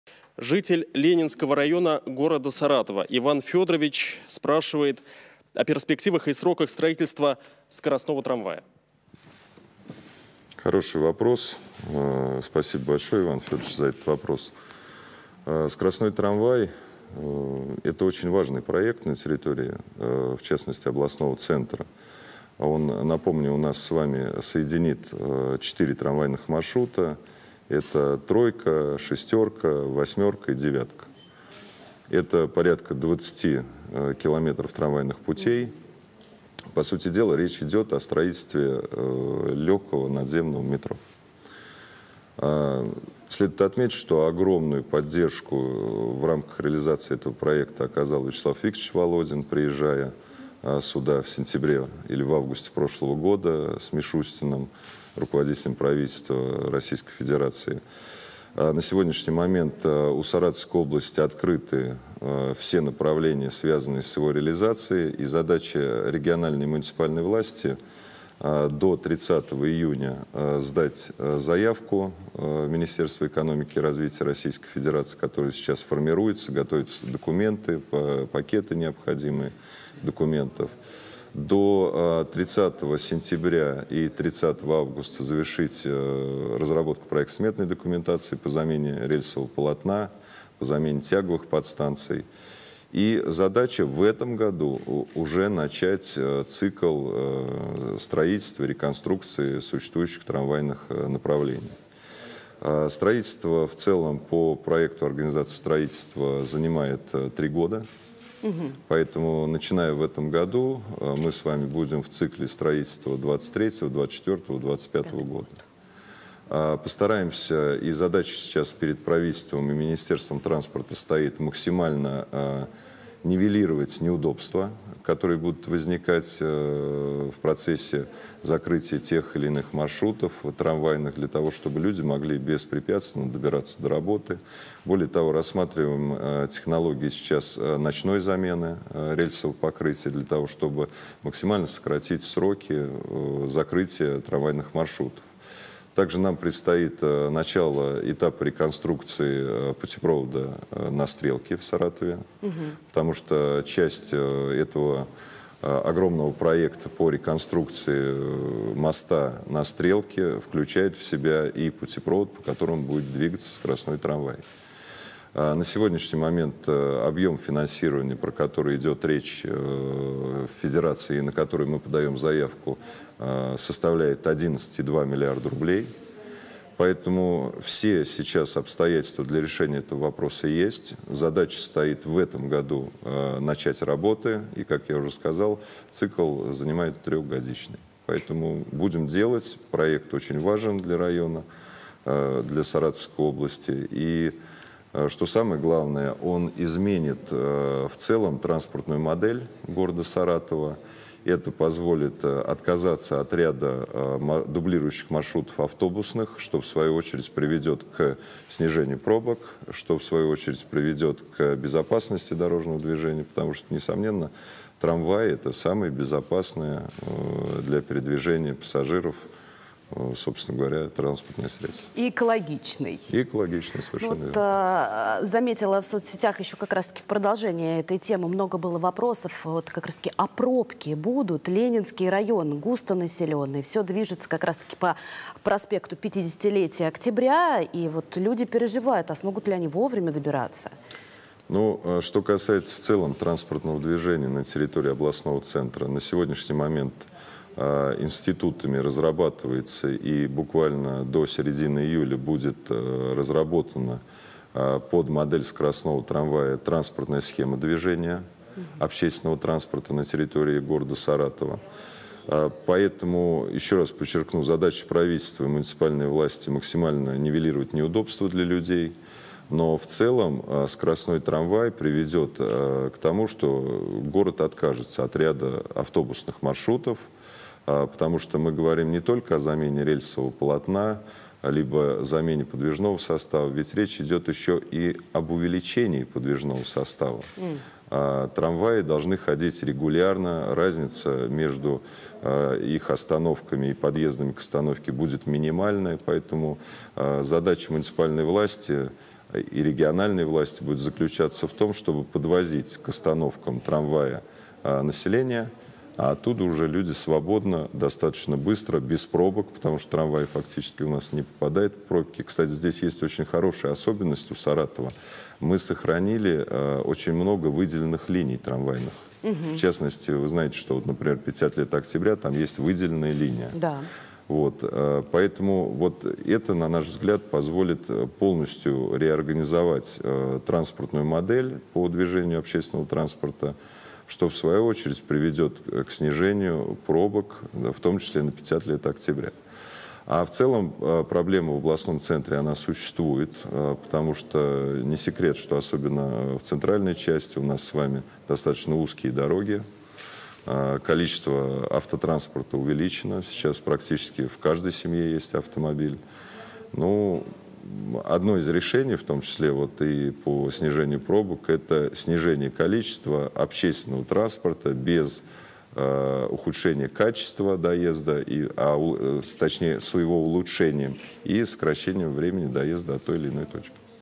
Роман Бусаргин анонсировал открытие наземного метро в Саратове
"По сути дела, речь идет о строительстве легкого наземного метро. У Саратовской области открыты все направления, связанные с реализацией проекта. Стоит задача до 30 июня сдать заявку в Министерство экономического развития РФ.

 До 30 сентября и 30 августа надо завершить разработку проектно-сметной документации по замене рельсового полотна и тяговой подстанции. В этом году мы должны начать цикл строительства и реконструкции существующих трамвайных направления.  Подробнее на ИА "Взгляд-инфо"